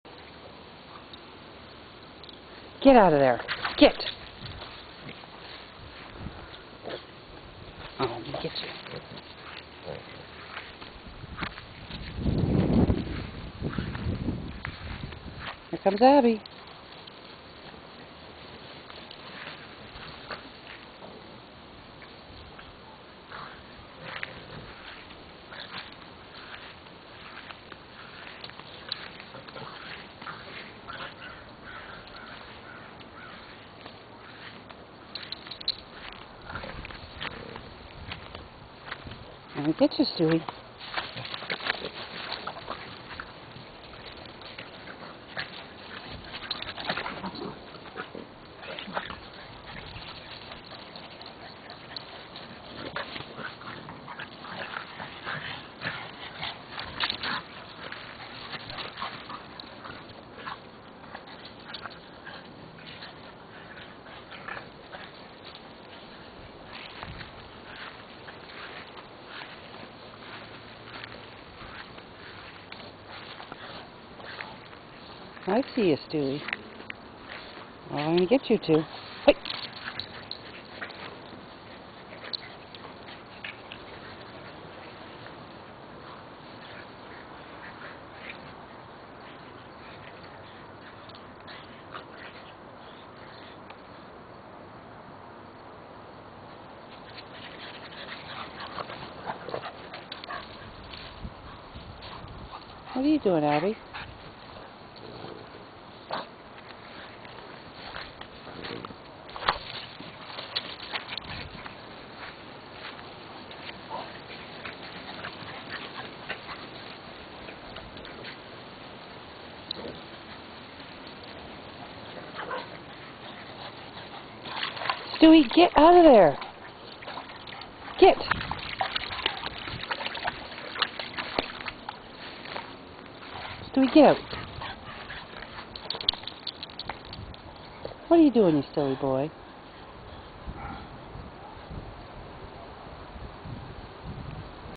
Stewie and Abby in the yard
Just Stewie and Abby enjoying their new yard with Stewie in and out of the pond.